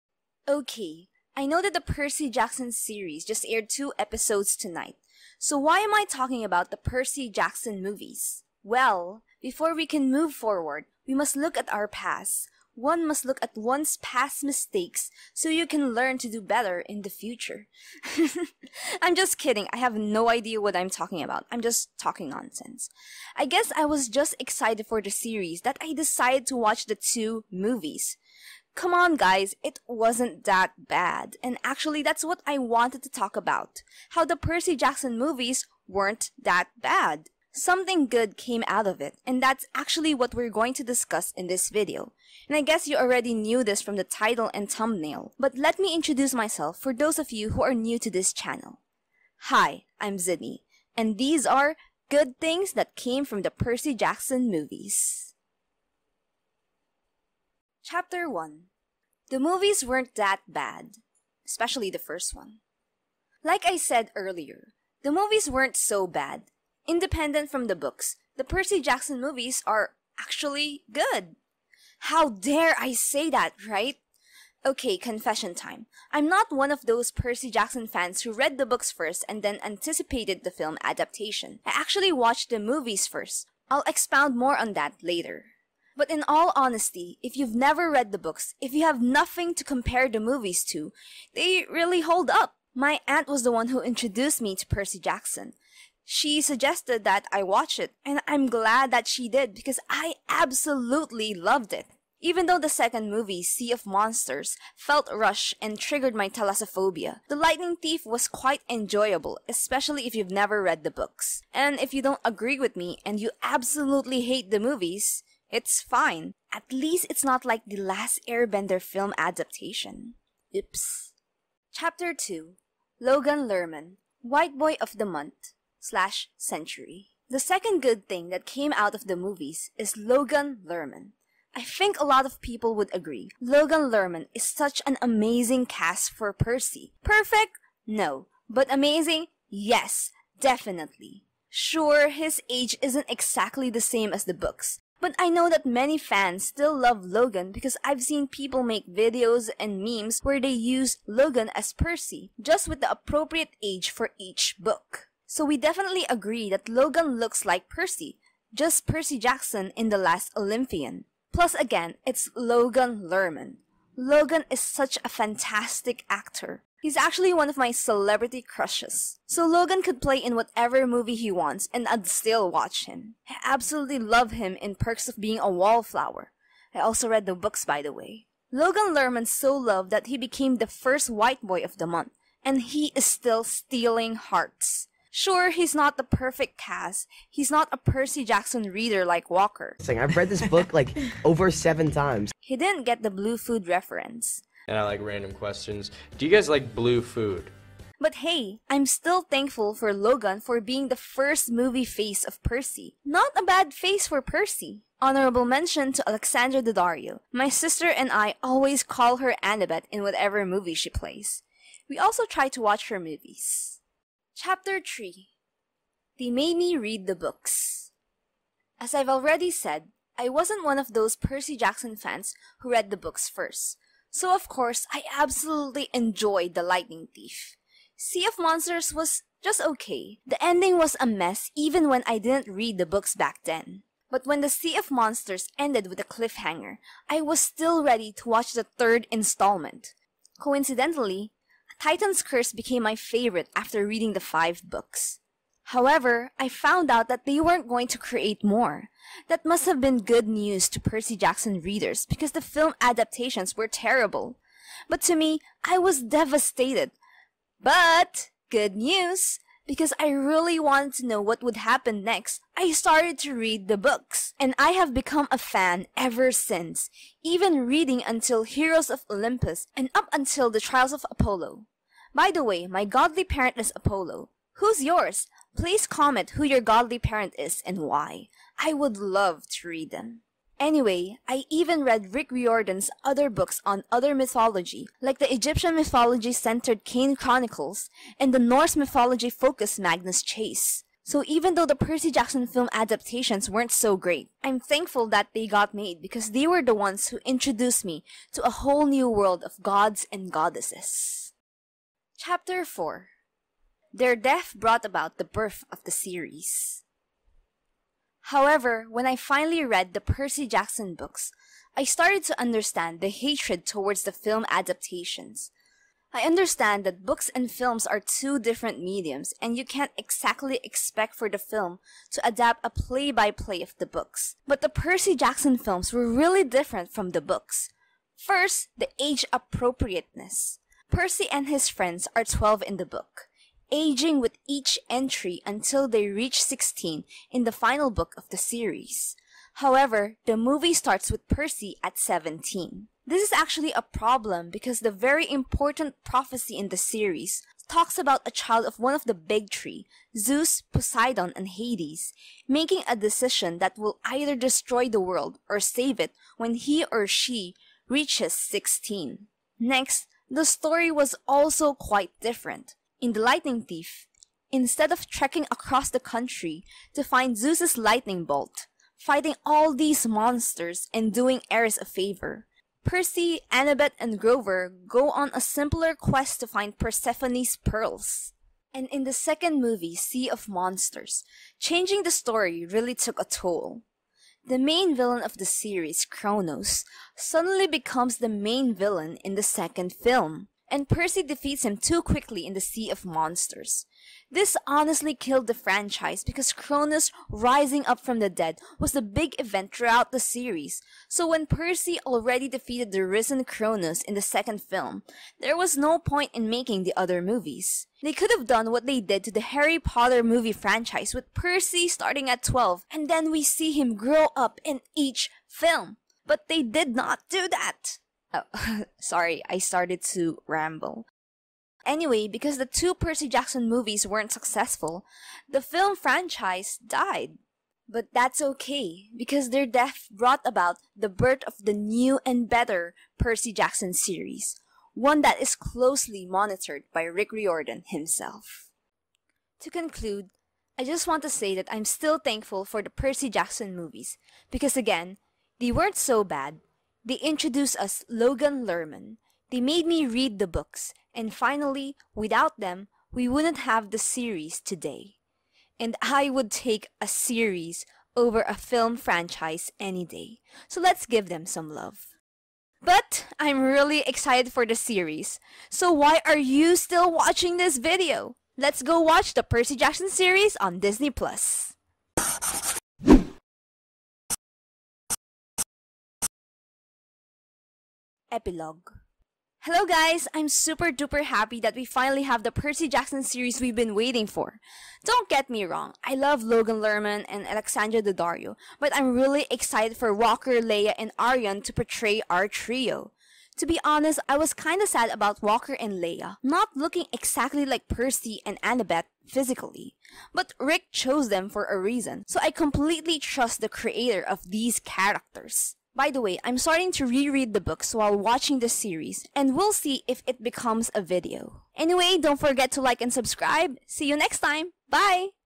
Good Things That Came from The Percy Jackson Movies
I know that the Percy Jackson series is finally on Disney plus but I just wanted to look back on the film adaptations, and I realized that they weren't so bad! Something good came out of them and that's what I will be talking about in this video.